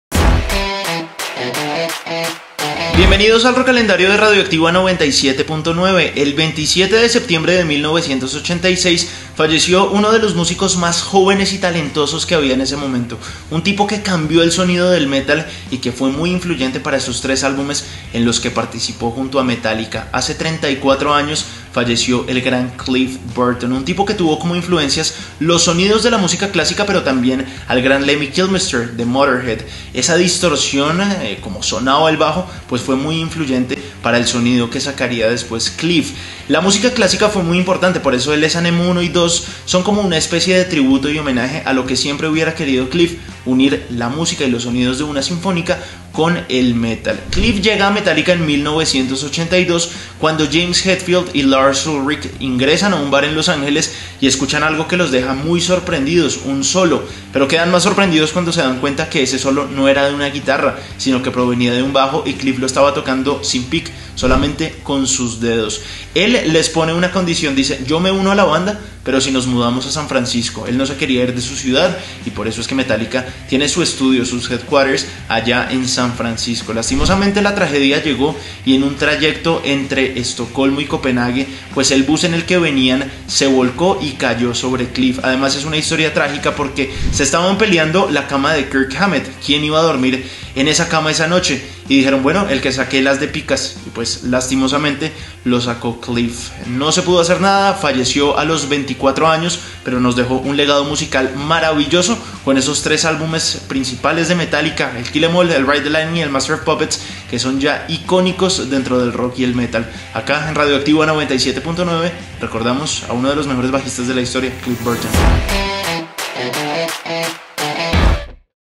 Rockalendario | 34 años sin Cliff Burton
Este 27 de septiembre se cumplen 34 años sin Cliff Burton, el bajista de Metallica desde 1982 hasta el día de su muerte en 1986.